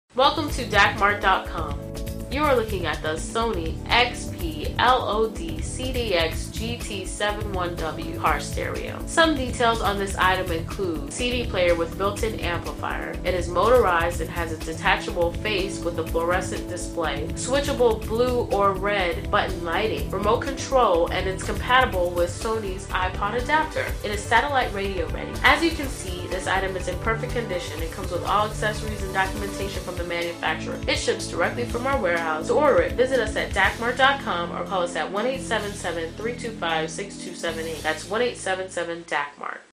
Sony Xplod Cdx-Gt71W Fm Cd Mp3 Wma Car Stereo - CdxGt71W
You are looking at the Sony Xplod Cdx-Gt71W Fm Cd Mp3 Wma Car Stereo

Some details on this item include:

-CD player with built-in amplifier (17 watts RMS/52 peak x 4 channels)
-Plays CDs, CD-Rs, and CD-RW discs, including discs loaded with MP3, WMA, AAC, and ATRAC3plus music files (except Digital
-Rights-protected files)
-Motorized, detachable face with a fluorescent display
-Switchable blue or red button lighting
-Remote control
-Compatible with Sony''s iPod adapter
-Satellite radio-ready (XM or SIRIUS tuner, subscription, and antenna required)